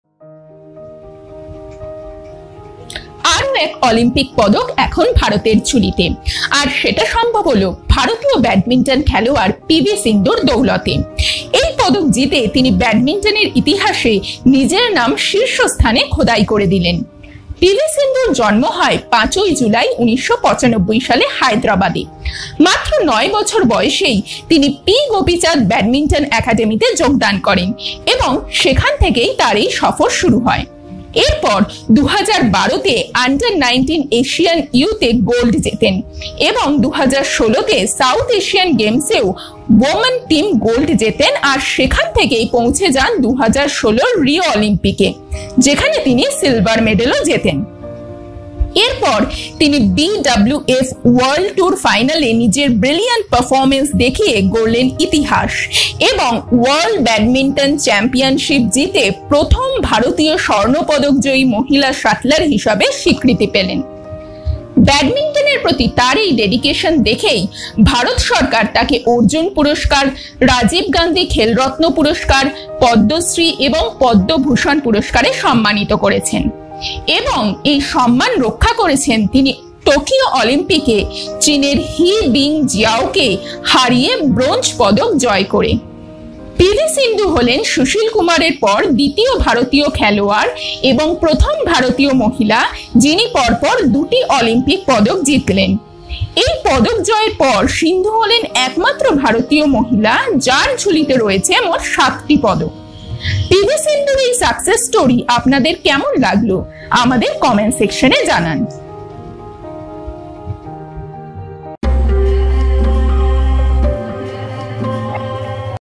P V Sindhu won Bronze Medal in Tokyo Olympic 2020 | Badminton Women Single
P V Sindhu won Bronze Medal in Tokyo Olympic 2020 | Badminton Women Single | Motivation and Inspiration |

প্রেরণার গল্প
অনুপ্রেরণার গল্প
inspiring story
motivational story